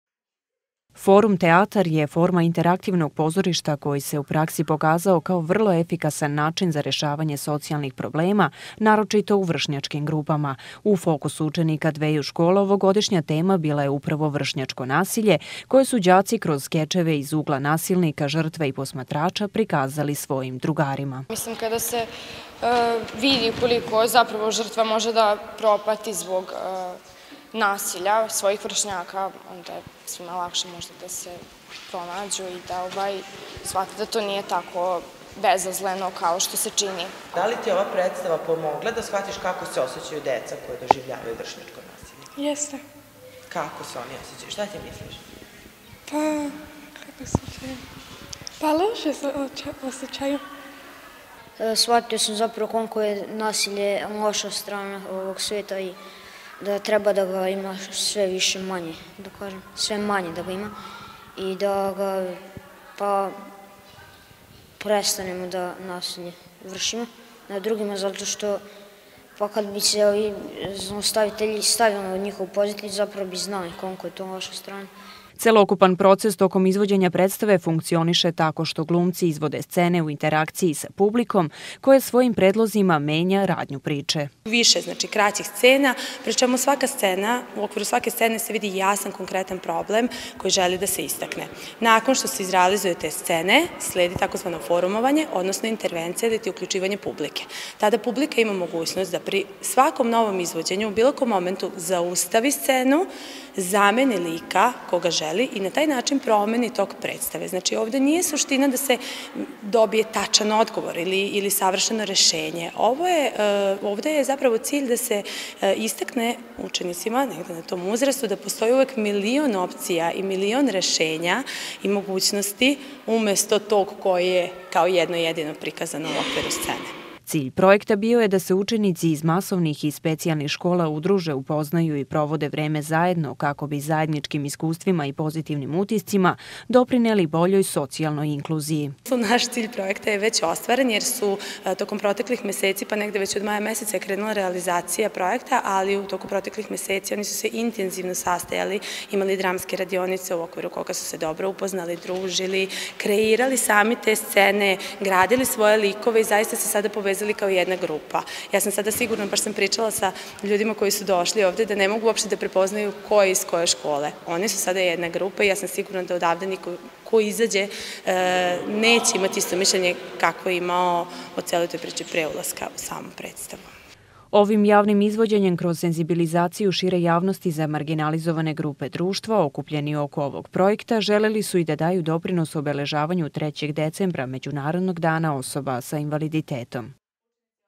Kroz teatar do bolje socijalne inkluzije i smanjenja vršnjačkog nasilja
Udruženje „ZaJedno“ u maju mesecu počelo je sa realizacijom projekta „Inkluzivni Forum teatar“ u kome su učestvovali učenici viših razreda OŠ „Ivan Goran Kovačić“ i Školskog centra sa domom učenika „Dositej Obradović“, a čiji rezultat je danas prikazan u vidu predstave koje su đaci izveli za svoje vršnjake.

Forum teatar je forma interaktivnog pozorišta, koji se u praksi pokazao kao vrlo efikasan način za rešavanja socijalnih problema, naročito u vršnjačkim glupama. U fokusu učenika dveju škola, ovogodišnja tema bila je upravo vršnjačko nasilje, koje su đaci kroz skečeve iz ugla nasilnika, žrtve i posmatrača prikazali svojim drugarima.

Celokupan proces tokom izvođenja predstave funkcioniše tako što glumci izvode scene u interakciji sa publikom, koja svojim predlozima menja radnju priče.

Cilj projekta bio je da se učenici iz masovnih i specijalnih škola udruže, upoznaju i provode vreme zajedno, kako bi zajedničkim iskustvima i pozitivnim utiscima doprineli boljoj socijalnoj inkluziji.

Ovim javnim izvođenjem kroz senzibilizaciju šire javnosti za marginalizovane grupe društva, okupljeni oko ovog projekta želeli su i da daju doprinos obeležavanju 3. decembra - Međunarodnog dana osoba sa invaliditetom.